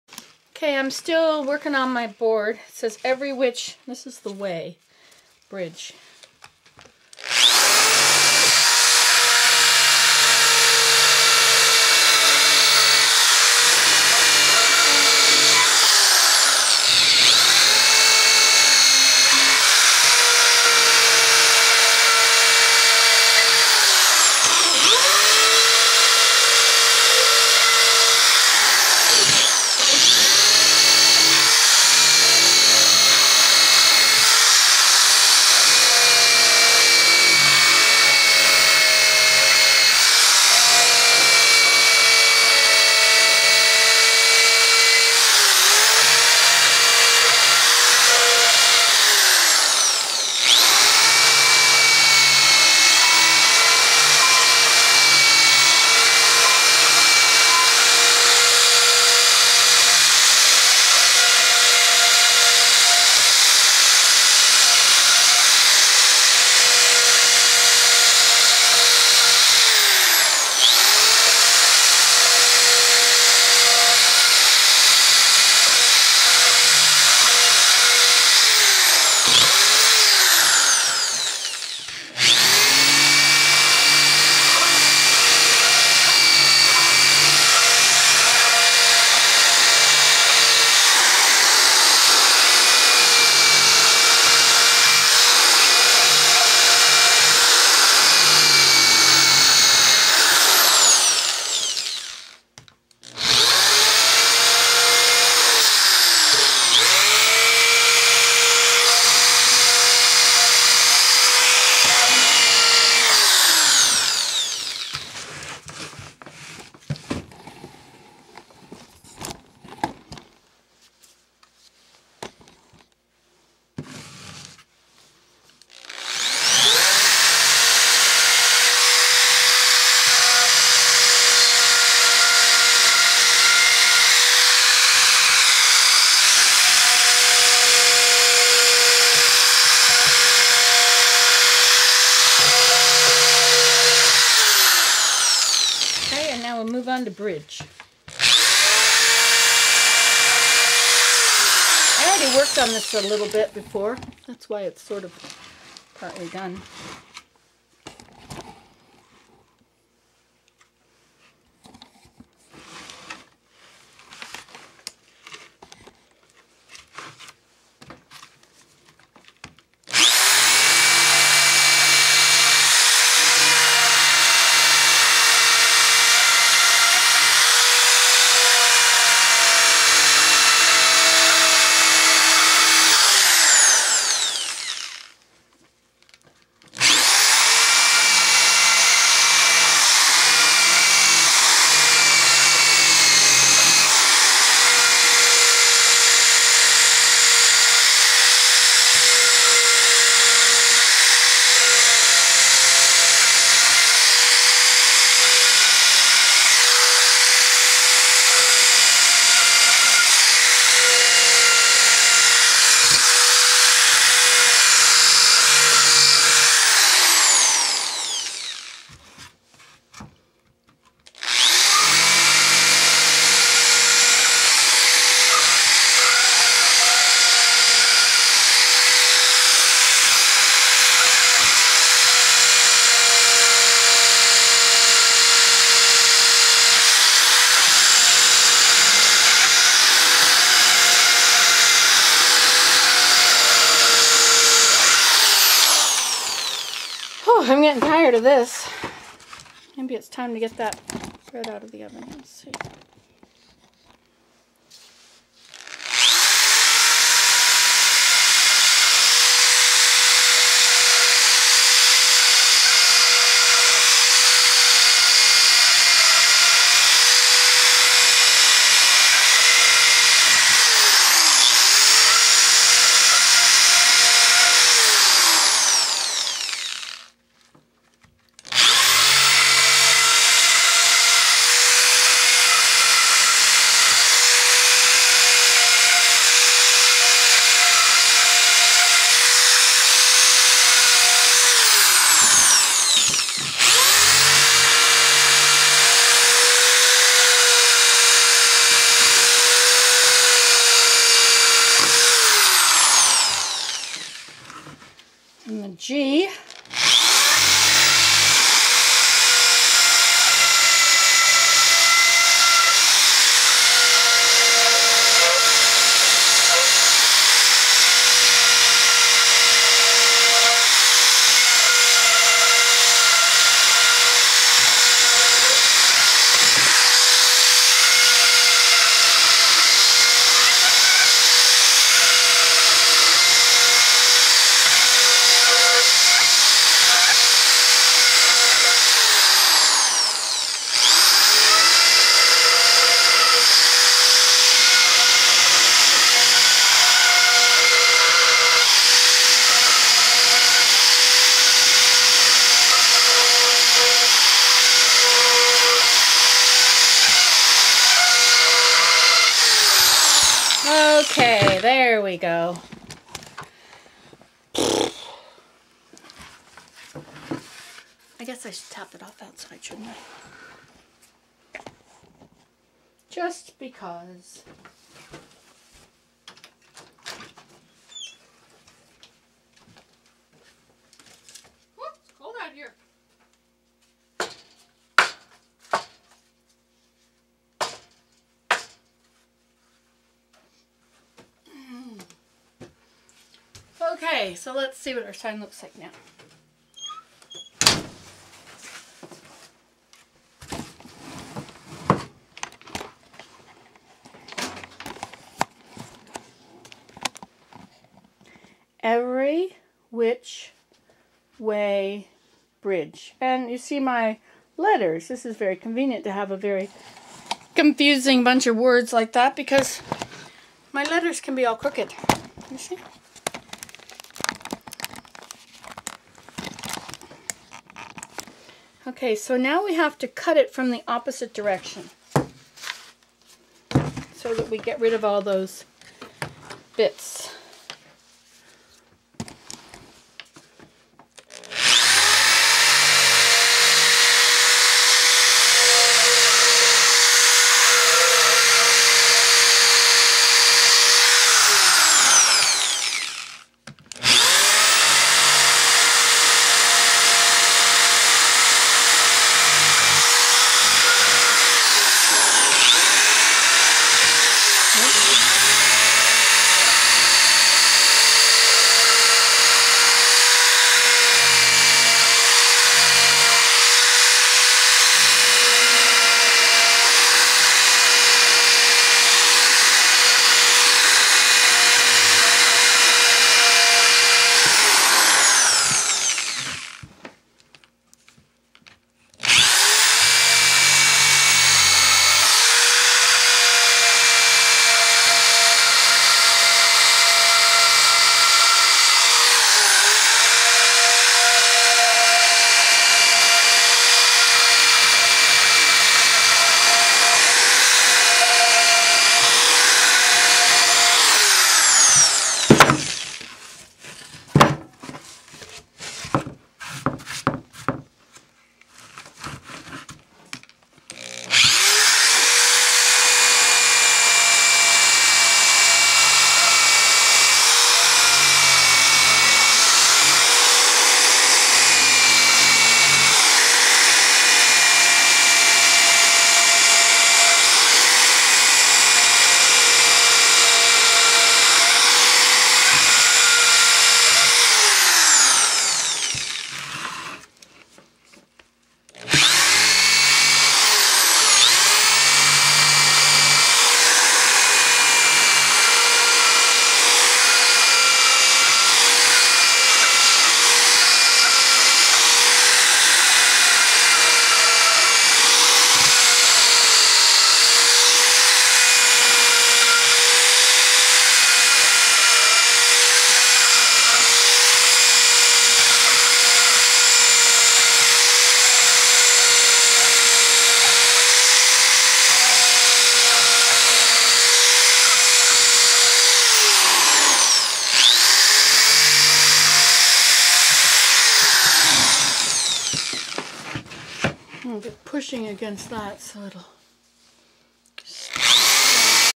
Nancy Today: Part 2- Every Which Way Bridge ASMR
Working on my sign for every which way bridge